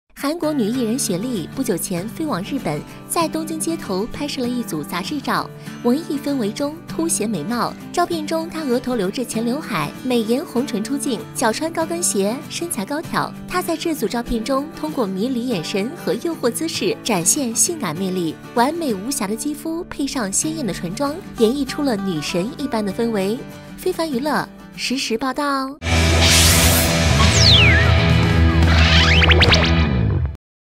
【近期】崔雪莉東京街頭寫真流出 “油頭”造型女人味十足！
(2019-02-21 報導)

南韓女藝人雪莉不久前飛往日本，在東京街頭拍攝了一組雜誌照。